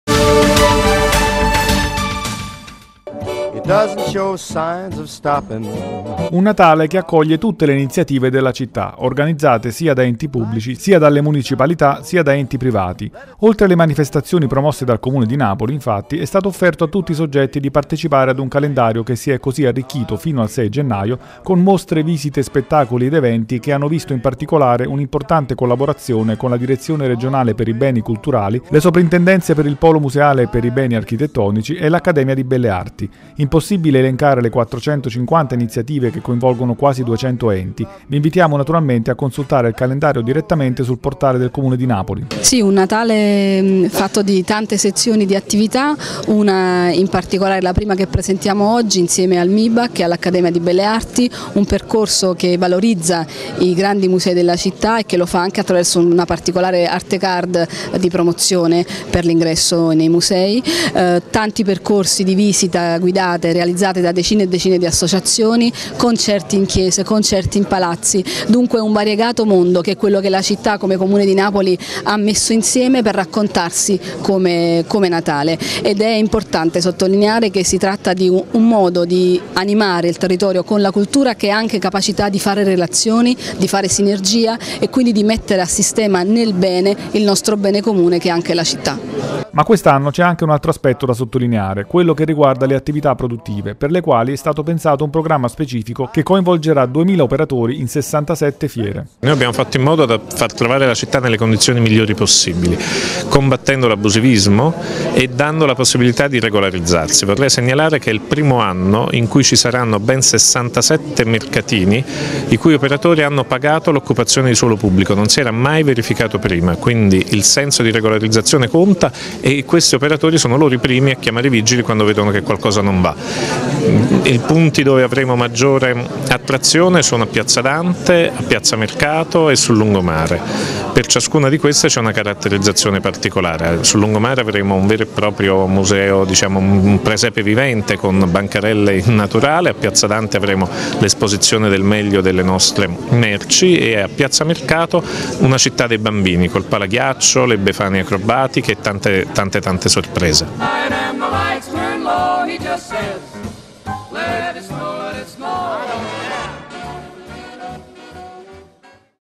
2012, il Natale di tutti
Comune di Napoli, enti pubblici, municipalità, enti privati: il Natale accoglie tutte le iniziative della città, in 450 mostre, visite, spettacoli ed eventi, oltre ad uno speciale programma per le attività produttive che coinvolgerà 2000 operatori in 67 fiere.